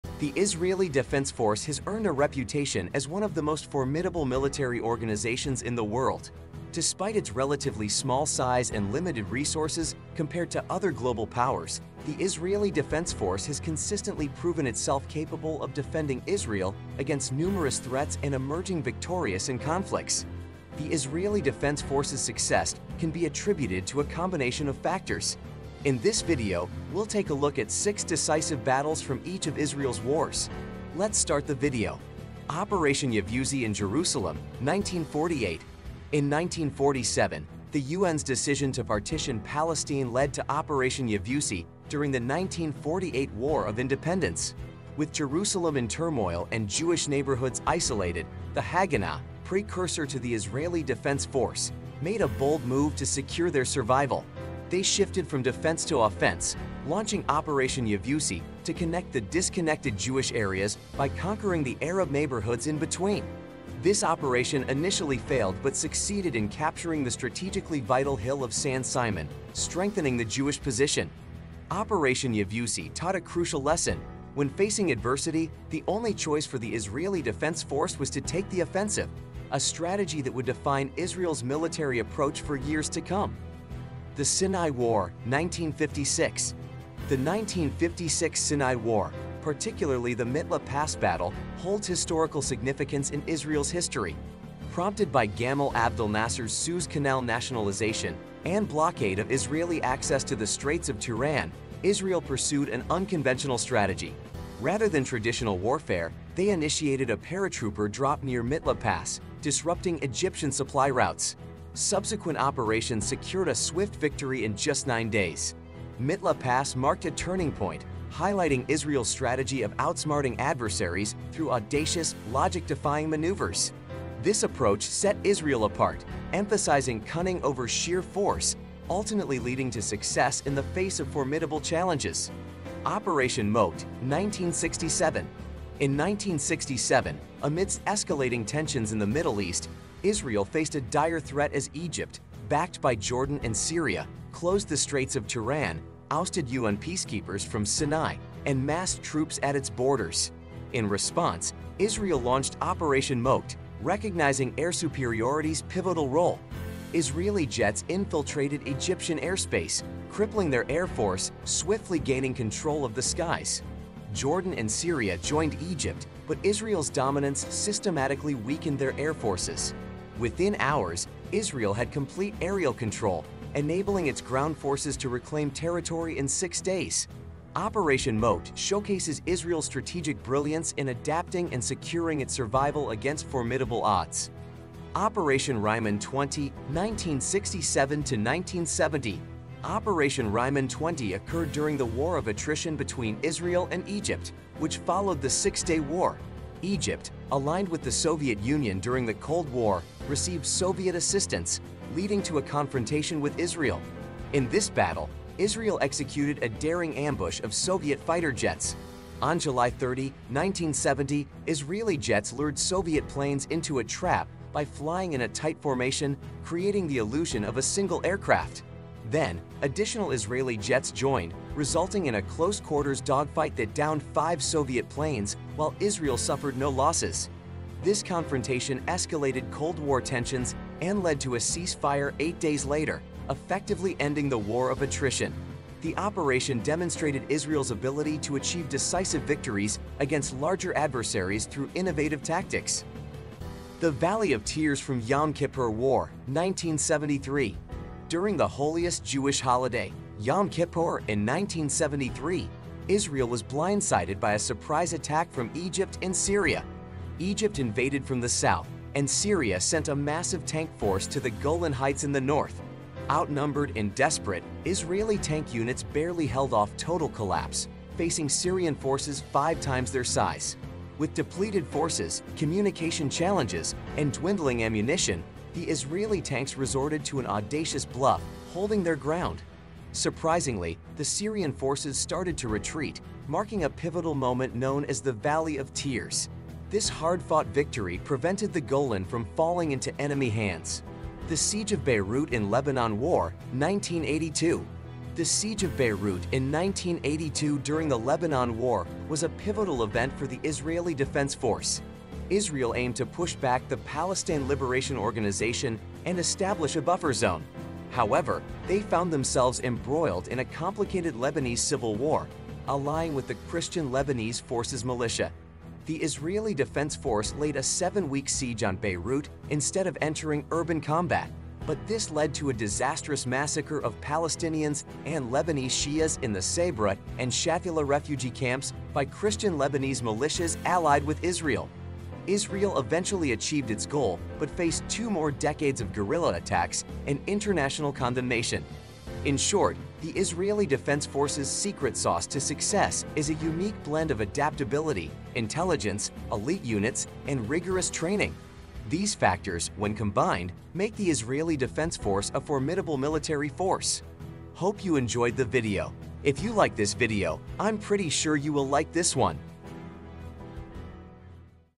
Why Israel never lost a battle!
The Israeli Defence Force has earned a reputation as one of the most formidable military organizations in the world. Despite its relatively small size and limited resources compared to other global powers, the Israeli Defence Force has consistently proven itself capable of defending Israel against numerous threats and emerging victorious in conflicts. The Israeli Defense Force's success can be attributed to a combination of factors. In this video, we’ll take a look at six decisive battles from each of Israel's wars. Let’s start the video.